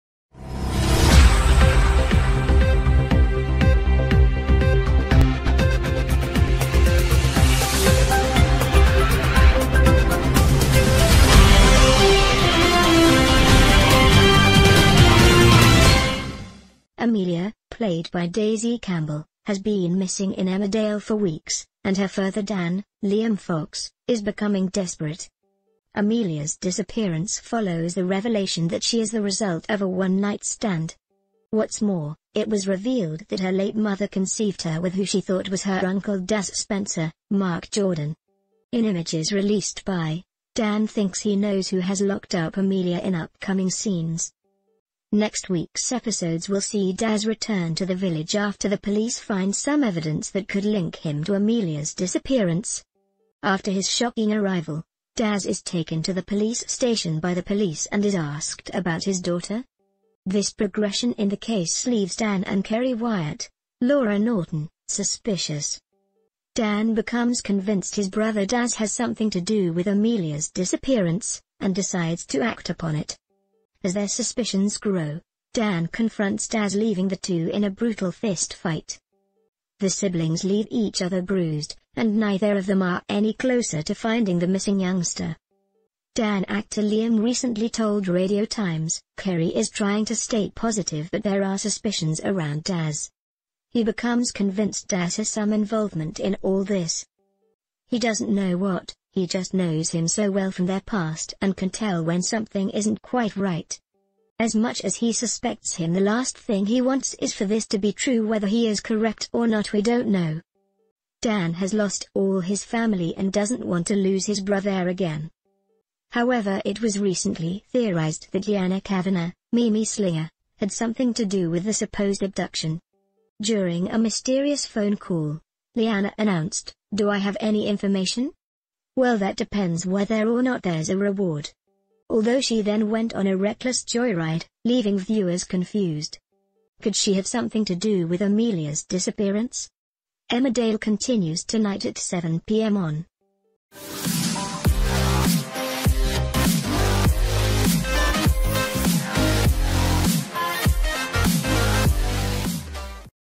Emmerdale spoilers: Daz Spencer revealed as Amelia's kidnapper? Star spills all
Amelia (played by Daisy Campbell) has been missing in Emmerdale for weeks, and her father Dan (Liam Fox) is becoming desperate.
Amelia’s disappearance follows the revelation that she is the result of